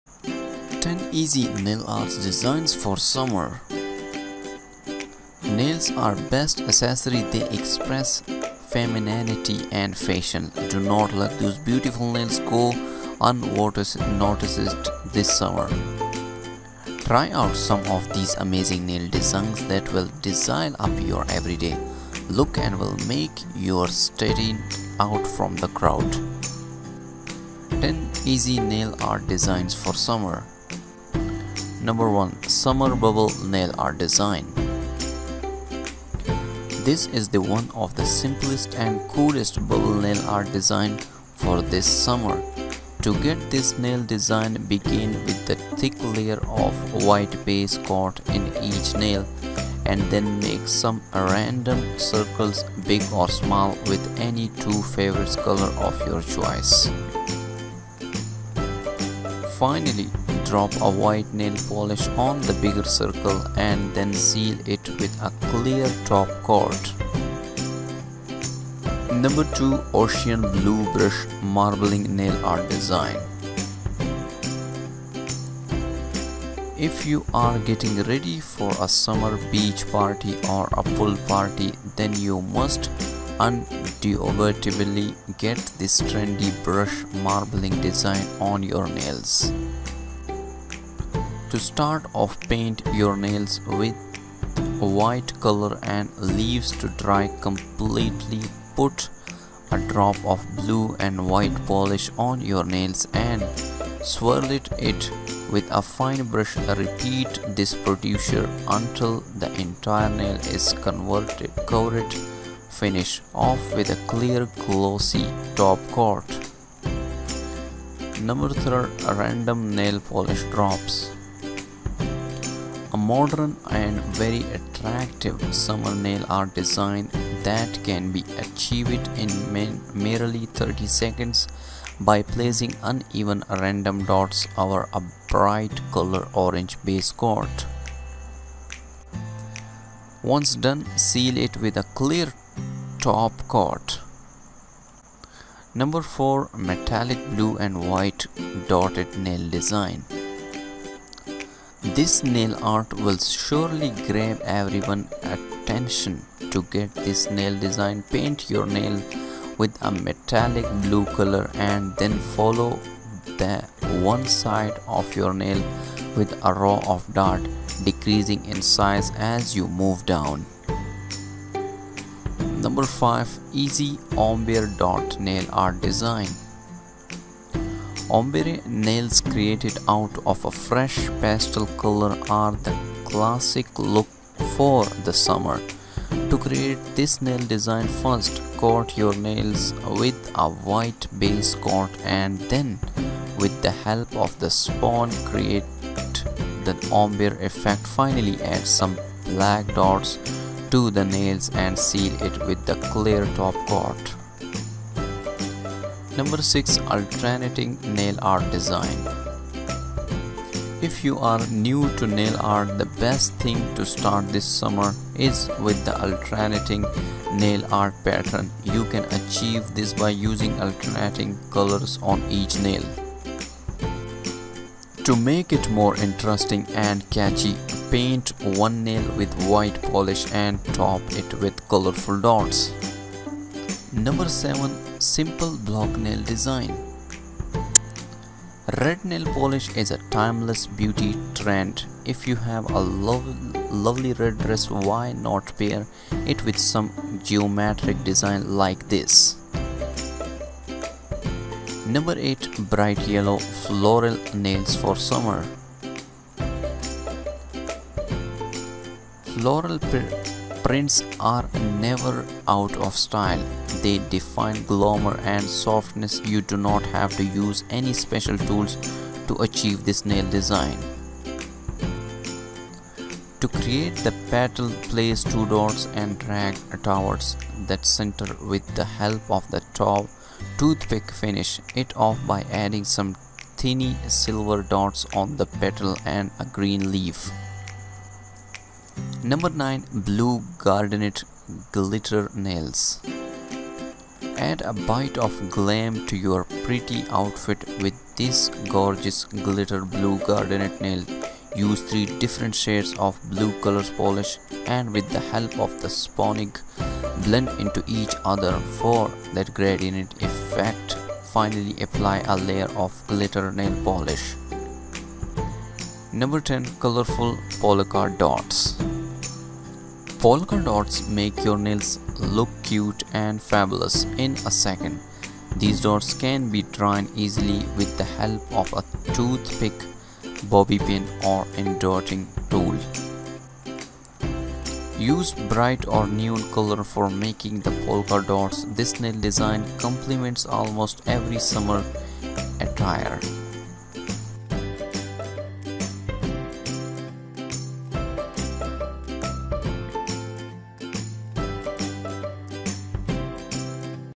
10 Easy Nail Art Designs For Summer
10 Easy Nail Art Designs For Summer

Nails are your best accessory.They express femininity and fashion. Don’t let those beautiful nails go unnoticed this summer.Try out some of these amazing nail designs that will dazzle up your everyday look and will make you stand out from the crowd.

10 Easy Nail Art Designs to Rock this Summer

1.Summer Bubble Nail Art Design

This is one of the simplest and coolest bubble nail art designs for the summer.To get this nail design, begin with a thick layer of white base coat on each nail and then make some random circles (big & small) with any two favorite colors of your choice.Finally, drop a white nail polish on the bigger circles and then seal it with a clear top coat.

2.Ocean Blue Brush Marbling Nail Art Design

If you are getting ready for a summer beach party or a pool party then you must undoubtedly get this trendy brush marbling design on your nails.To start off, paint your nails with white color and leave to dry completely.Put a drop of blue and white polish on your nail and swirled it with a fine brush, repeat this procedure until the entire nail is covered.Finish off with a clear glossy top coat.

3.Random Nail Polish Drops

A modern and very attractive summer nail art design that can be achieved in merely 30 seconds by placing uneven, random dots over a bright color orange base coat.Once done, seal it with a clear top coat.

4.Metallic Blue & White Dotted Nail Design

This nail art will surely grab everyone’s attention.To get this nail design paint your nails with a metallic blue color and then follow the one side of your nail with a row of dots (decreasing in size) as you move down.

5.Easy Ombre Dot Nail Art Design

Ombre nails created out of fresh pastel colors are the classic look for the summer.To create this nail design first, coat your nails with a white base coat and then with the help of a sponge create the ombre effect.Finally, add some black dots to the nails and seal it with a clear top coat.

6.Alternating Nail Art Design

If you’re new to nail art, the best thing to start this summer is with the alternating nail art pattern.You can achieve this by using alternating colors on each nail.To make it more interesting and catchy, paint one nail with white polish and top it with colorful dots.

7.Simple Block Nail Design

Red nail polish is a timeless beauty trend.if you have a lovely red dress why not pair it with some geometric designs like this.

8.Bright Yellow Floral Nails for Summer

Floral prints are never out of style.They define glamor and softness.You don’t have to use any special tools to achieve this nail design.To create the petals, place 2 dots and drag towards the center with the help of a toothpick.Finish it off by adding some tiny silver dots on the petals and a green leaf.

9.Blue Gradient Glitter Nails
Add a bit of glam to your party outfit with this gorgeous glitter blue gradient nails.Use 3 different shades of blue color polish and with the help of a sponge, blend into each other for that gradient effect.Finally, apply a layer of glitter nail polish.

10.Colourful Polka Dots

Polka dots make your nails look cute and fabulous in seconds. These dots can be drawn easily with the help of a toothpick, bobby pin or dotting tool.Use bright or neon colors for making the polka dots.This nail design complements almost every summer attire.